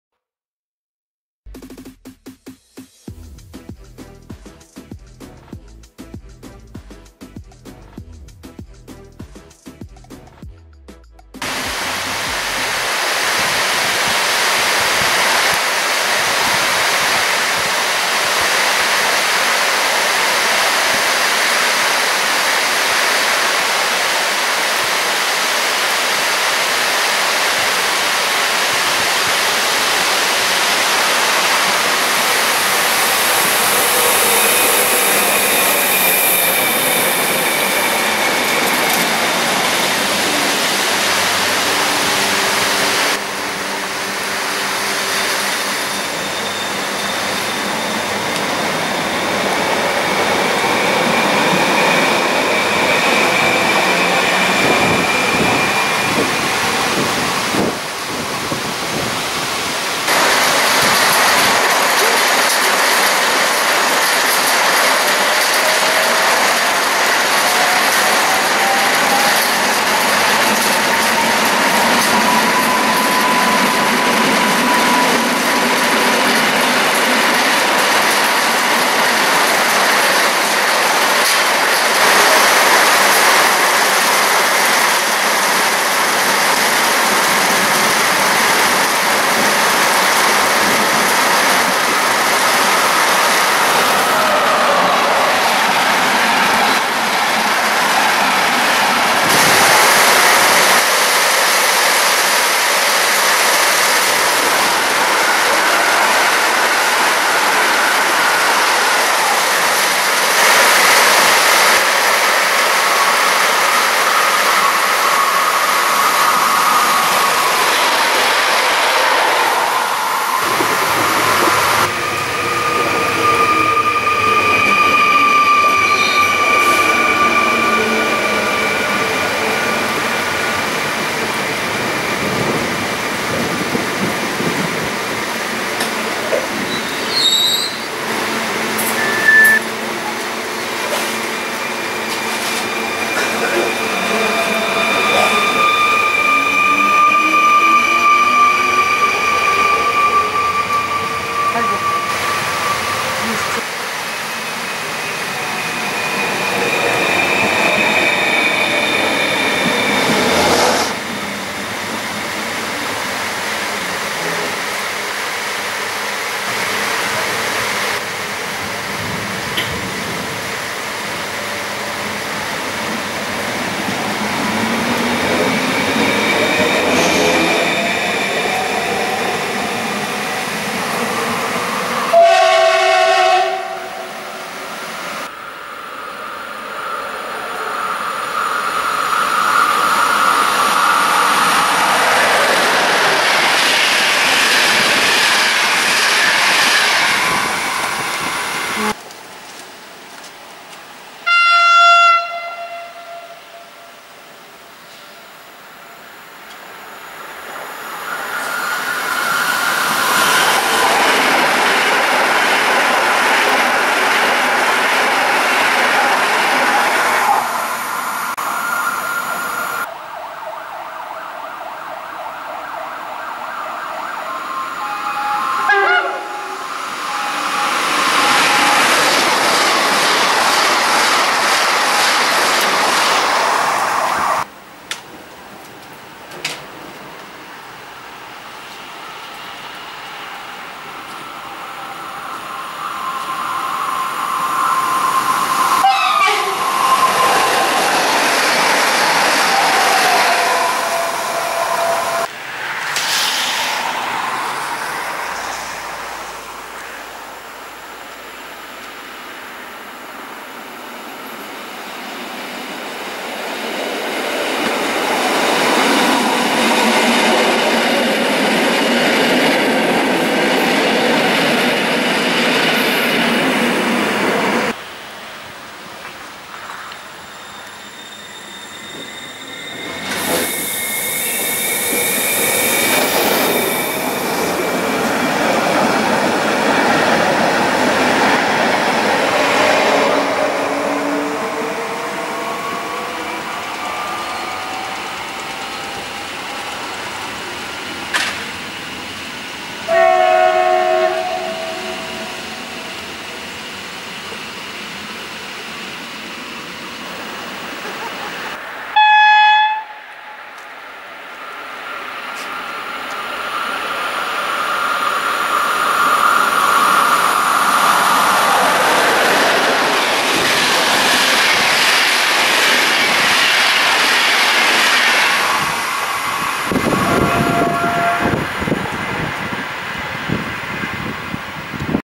Składanka kolejowa z p.o. Katowice Podlesie
Składanka kolejowa z przystanku osobowego Katowice Podlesie.

Składanka nagrywania 29.07.18  w godzinach: 15 - 18 wraz z YouTube-rem Train Spotter Mikołów - zapraszam na jego kanał! :)

- Informacje o składach wyświetlane są na bieżąco podczas filmu -